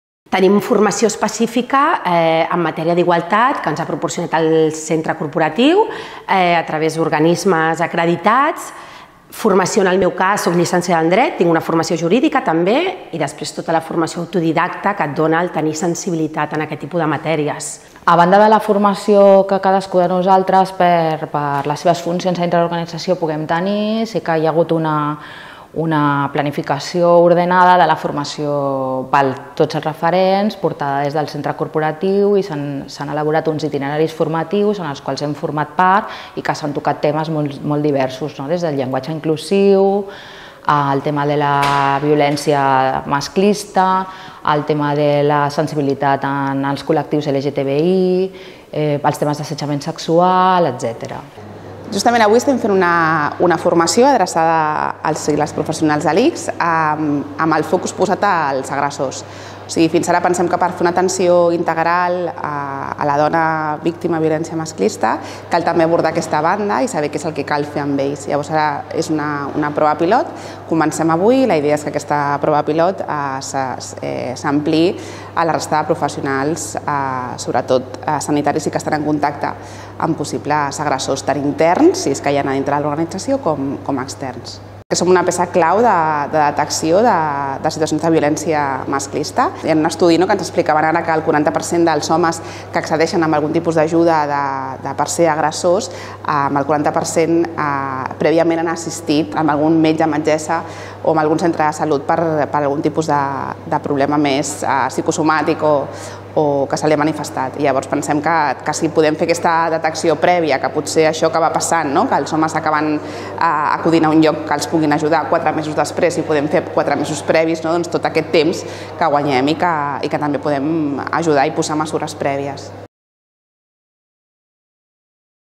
La formació, eina essencial per oferir una atenció de qualitat
Meritxell Herreros, directora de Persones i Organització i referent d’igualtat de l’Hospital de Viladecans, afegeix que aquests perfils intervenen en tres línies fonamentals com són la de facilitar totes les eines que tenen a la seva disposició per ajudar les persones que pateixen alguna problemàtica relacionada amb temes d’igualtat, sensibilitzar totes i tots els professionals així com les víctimes mateixes des d’una perspectiva de gènere, i garantir el compliment a tot el territori de les polítiques d’igualtat.

Per fer-ho, totes i tots els referents han hagut de fer una formació específica, a banda dels estudis i perfils professionals que puguin tenir. “Nosaltres compaginem aquestes tasques amb la nostra feina habitual i, per tant, hem d’alliberar algunes de les nostres hores per dedicar-nos plenament a les tasques d’igualtat”, comenta Lozano.